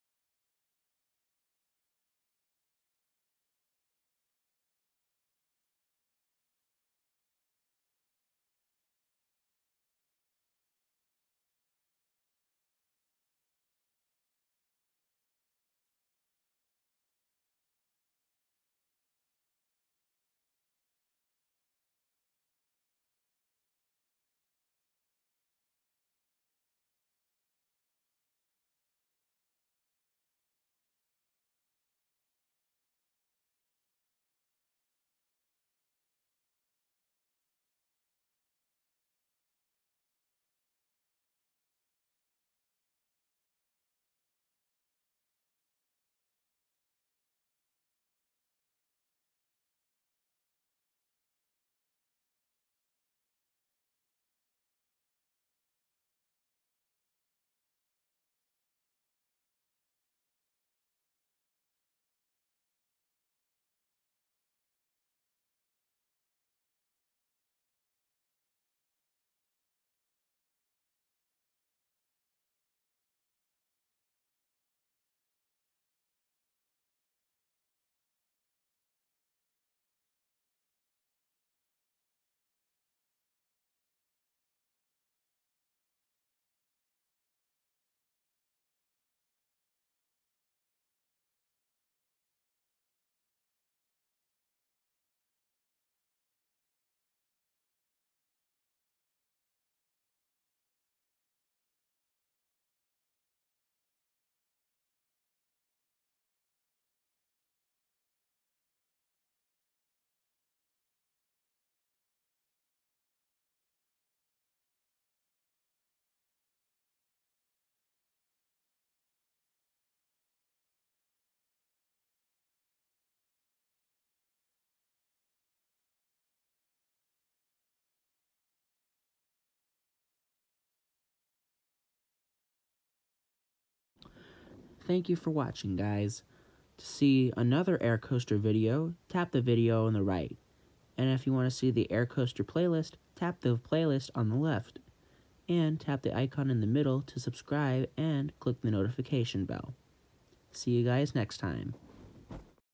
AirCoaster Episode 12 - Red Force
This is a new inverted coaster I’ve made called Red Force!